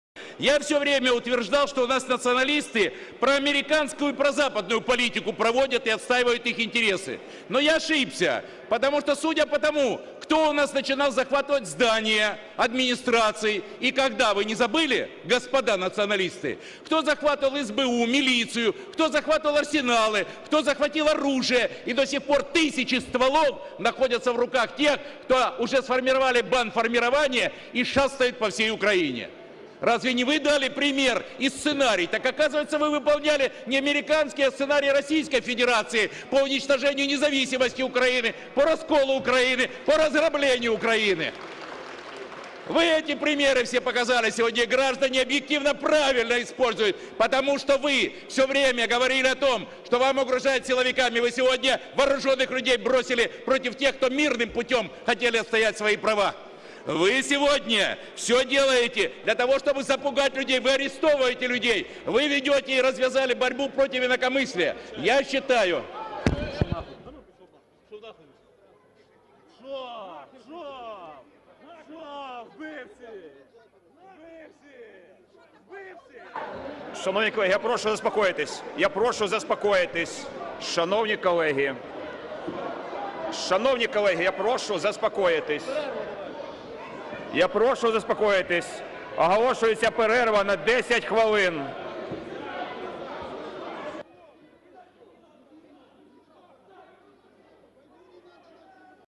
Welcome to Nulandistan: Svoboda Thugs Silence MP Petro Symonenko inside Ukraine's Parliament
Welcome to Nulandistan. Democracy flew out the door of Kiev in February 2014. Any semblances of a functioning democracy are disappearing day by day.

Brute force is being used against civilian protesters opposing the unelected Yatsenyuk regime that is backed by the US and the EU. Yatsenyuk's coup-installed regime has cracked down on media freedoms, sent the Ukrainian military to attack anti-coup protesters under the pretext of NATO-backed anti-terrorism operations, and started purges and witch hunts all over Ukraine.

The regime's officials have begun to glory Hitler's invasion of Ukraine and Eastern Europe too.

Inside the Verkhova Rada or Ukrainian Parliament any deputies who dare speak against the regime and its policies are silences and beaten up. This is the US-supported freedom of speech that has been brought to Ukraine, Nuland-style. In this video MP Petro Symonenko is pushed and prevented from speaking just for daring to condemn the use of the Ukrainian military against civilian protesters by Yatsenyuk's regime. Before being silenced, Symonenko made the key point of noting how the ultra-nationalists were serving foreign interests and not the national interests of Ukraine.

A atmosphere of intimidation and bullying is the new order of the day in Nulandistan and politicians opposing the regime are regularly threatened and beaten, some to the point of near death.

For more articles and information about the US-supported coup in Ukraine please visit Global Research